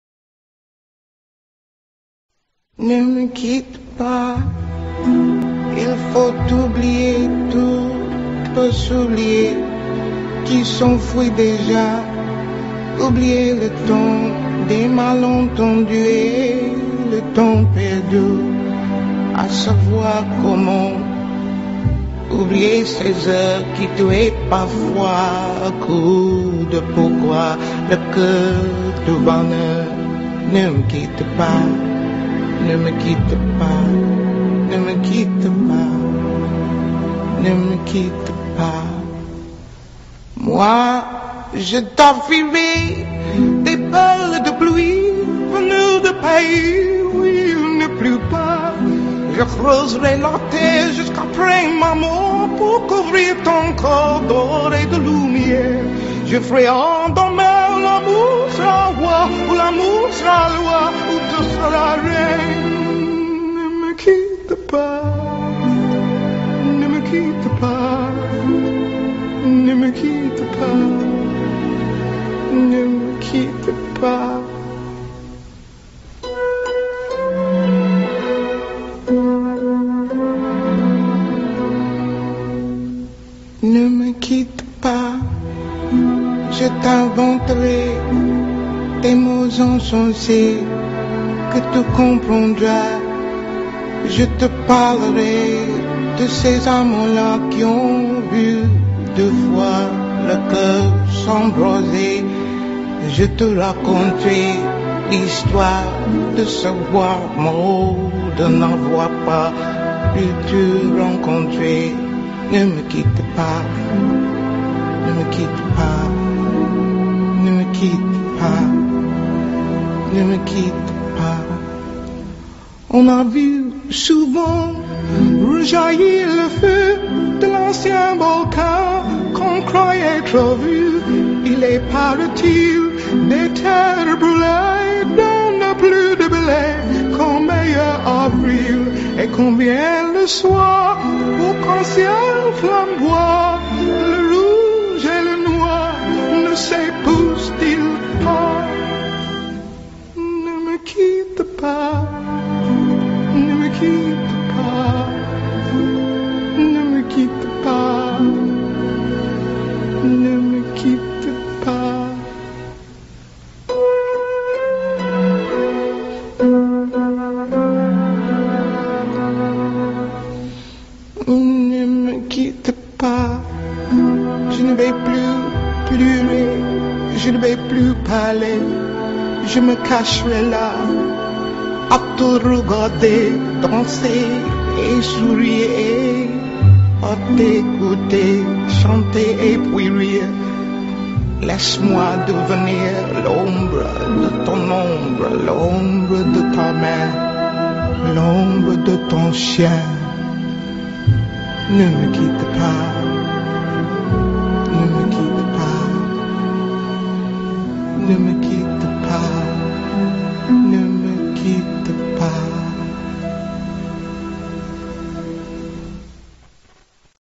Nina Simone Hommage - Painting by Zara Kand
Time-lapse oil painting of Nina Simone, by Zara Kand